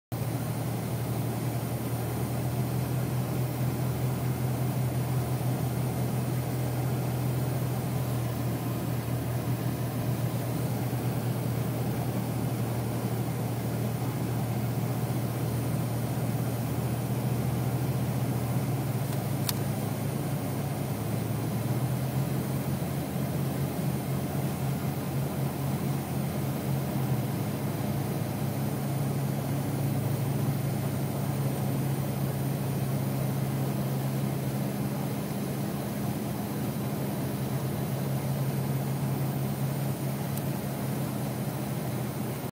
Brutal Canadian Winter Drive
On my way home from work. That car to the right looks familiar.